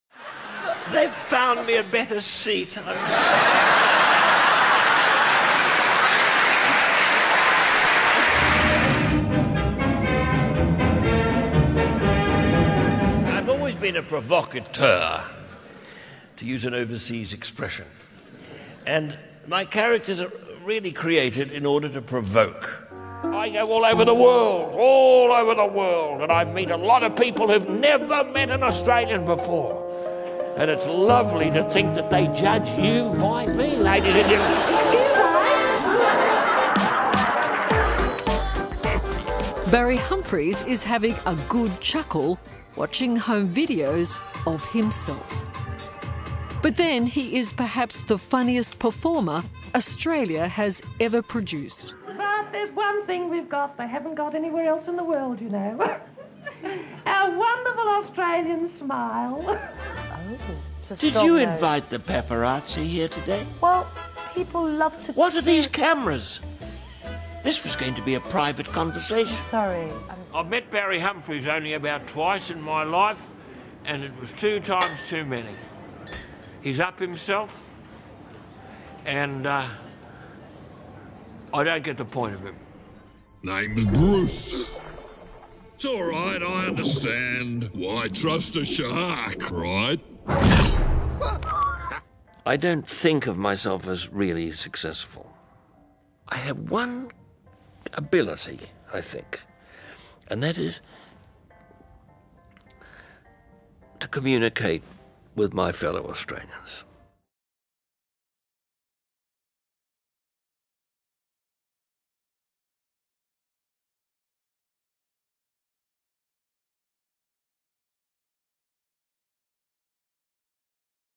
Morre intérprete da drag Dama Edna (Barry Humphries)
Canal The Guardian 22-04-23
Bem-vindo ao Canal oestadoacre! Acompanhe temas importantes do momento.